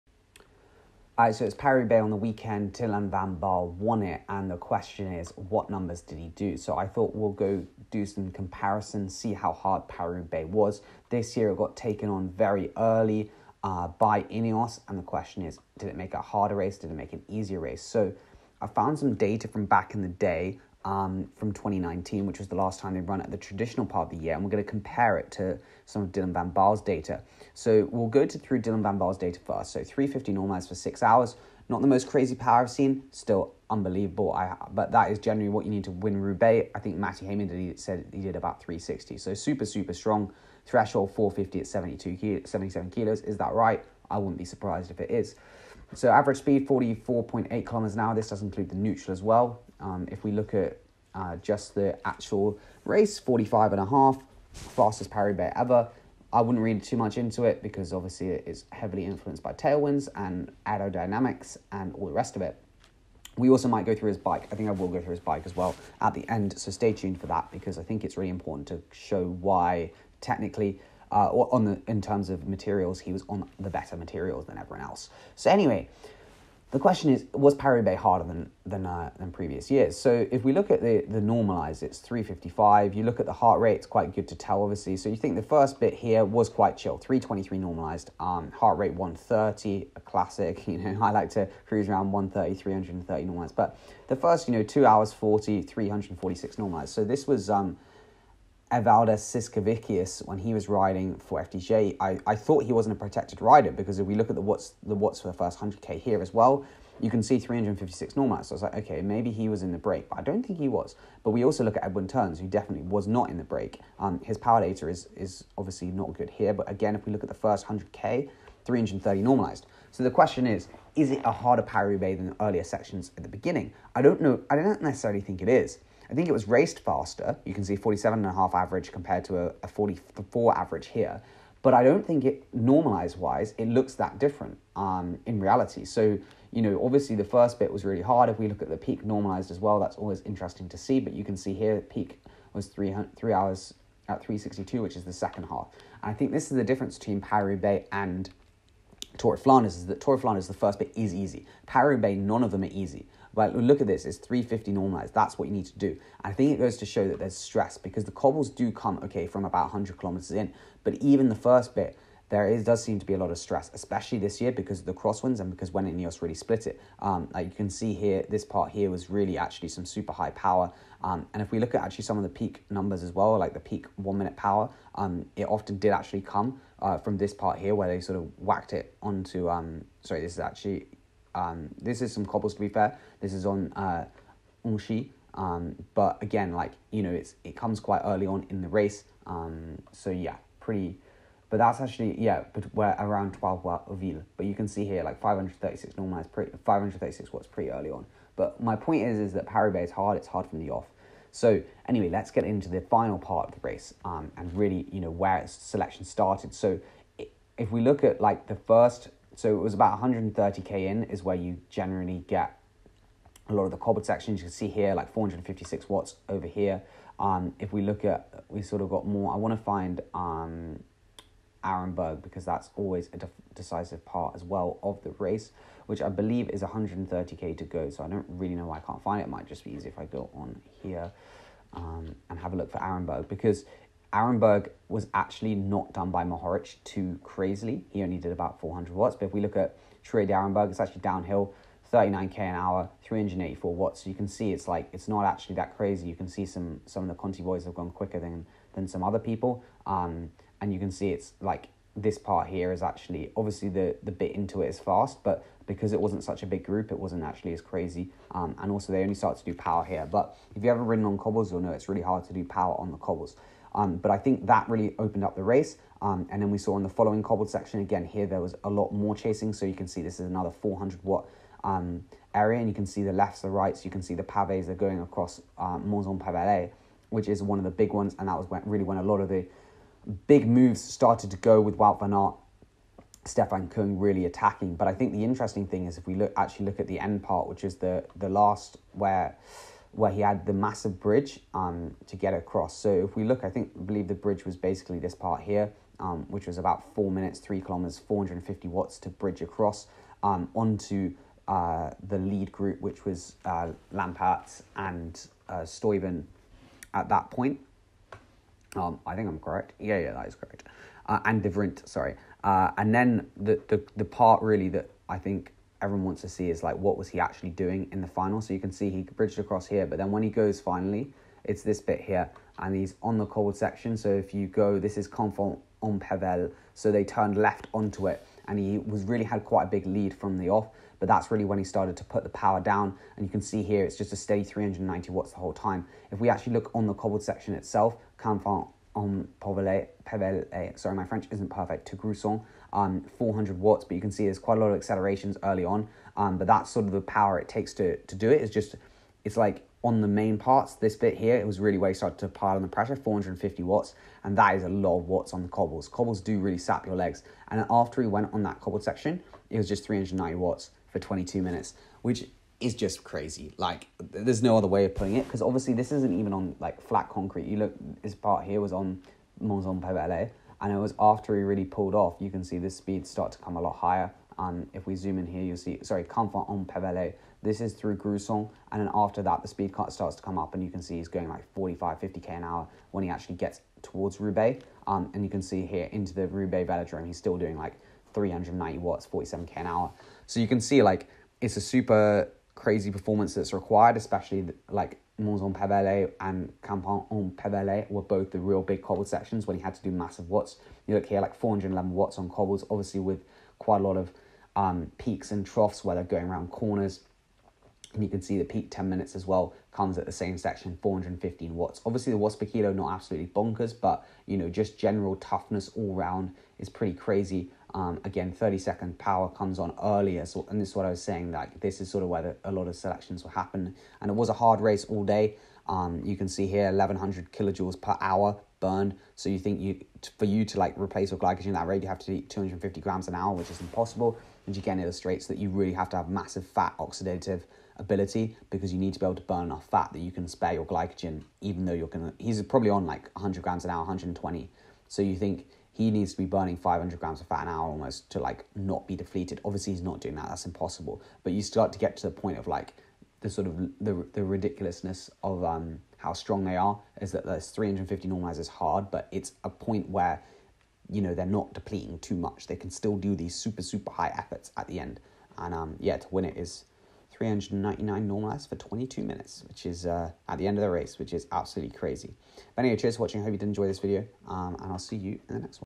VAN BAARLE ROUBAIX WIN - ANALYSED!!
I go through the power data of Dylan Van Baarle s win at paris roubaix 2022